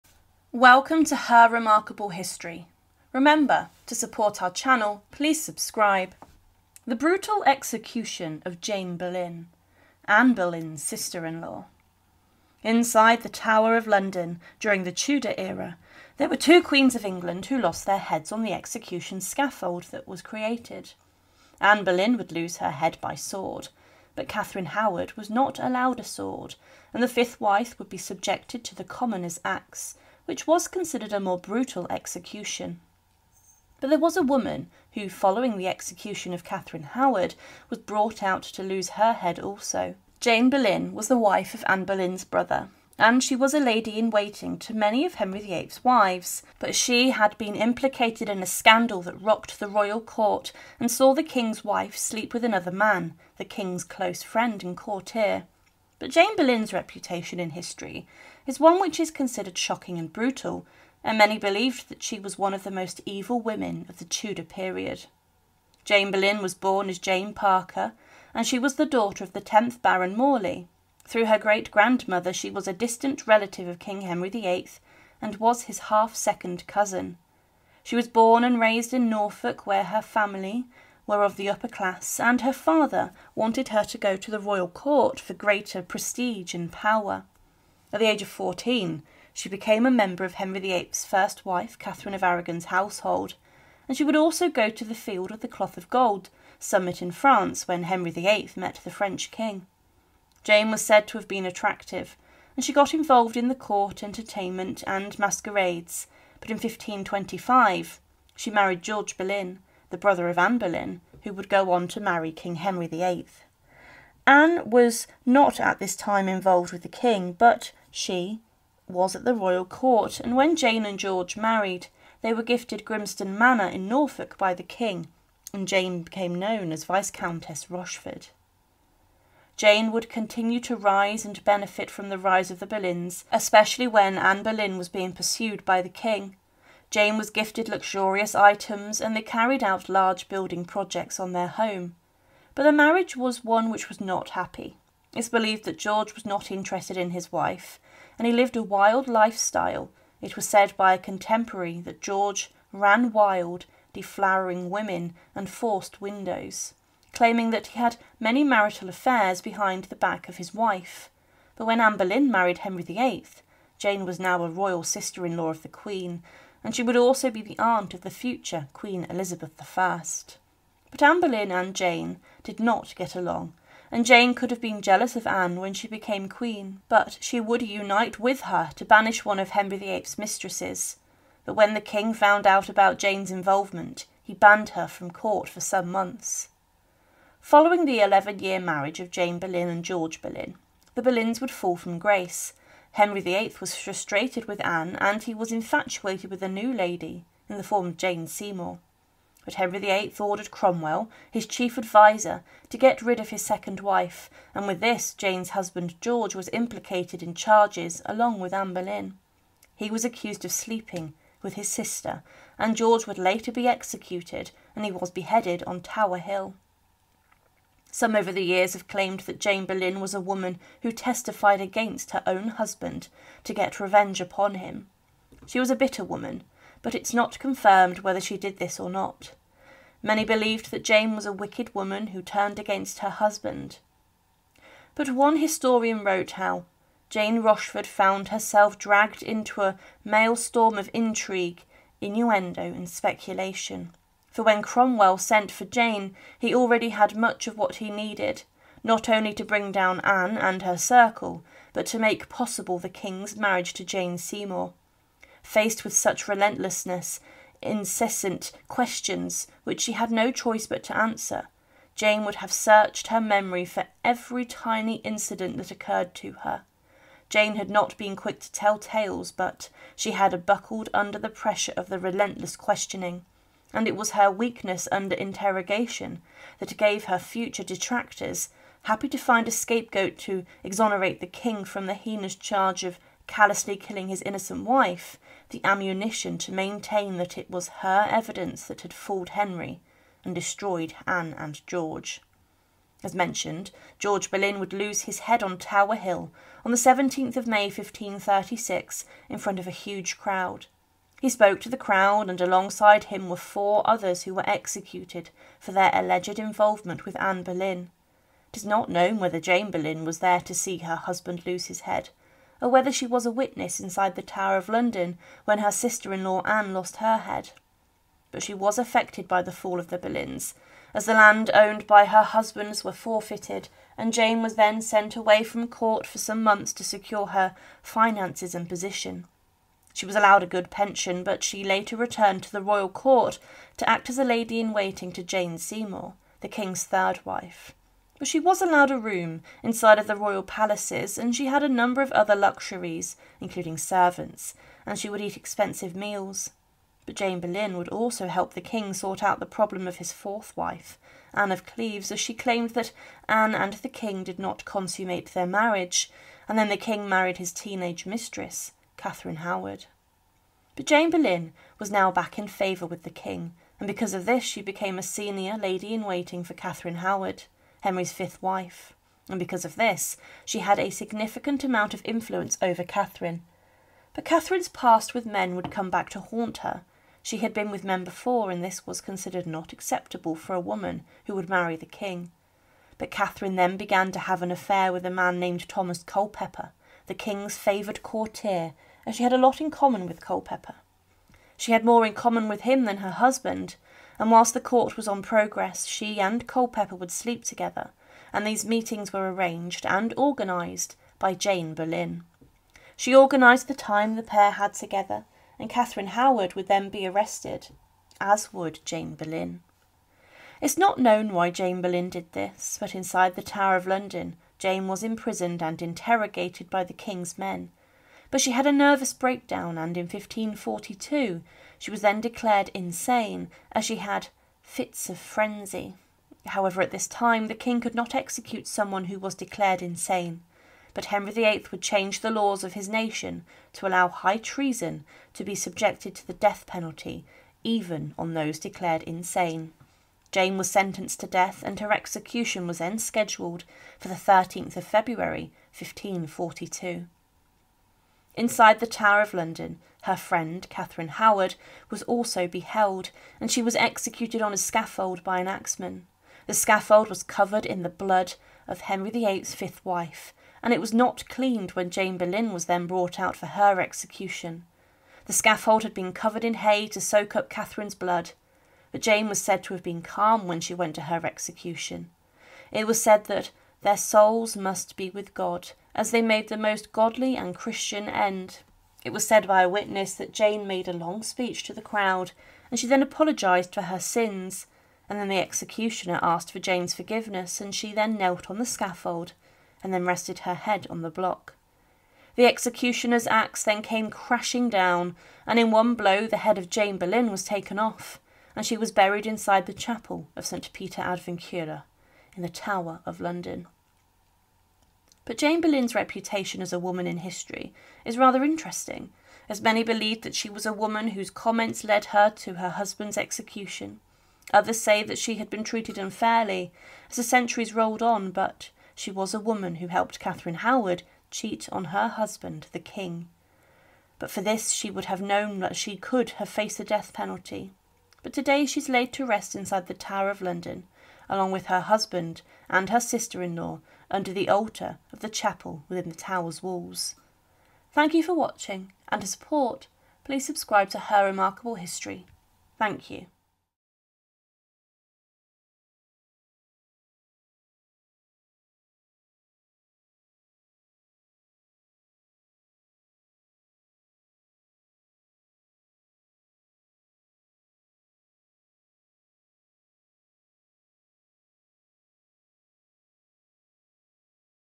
The BRUTAL EXECUTION Of Jane Boleyn - Anne Boleyn's sister in law
Inside of the Tower of London during the Tudor era, there were two queens of England who lost their heads on the execution scaffold that was created. Anne Boleyn would lose her head by sword, but Catherine Howard was not allowed a sword, and the fifth wife would be subjected to the commoner’s axe, which was considered a more brutal execution. But there was a woman who following the execution of Catherine Howard was brought out to lose her head also. Jane Boleyn was the wife of Anne Boleyn’s brother, and she was a lady in waiting to many of Henry VIII’s wives, but she had been implicated in a scandal that rocked the royal court, and saw the King’s wife sleep with another man, the King’s close friend and courtier. But Jane Boleyn’s reputation in History is one which is considered shocking and brutal, and many believe that she was one of the most evil women of the Tudor period.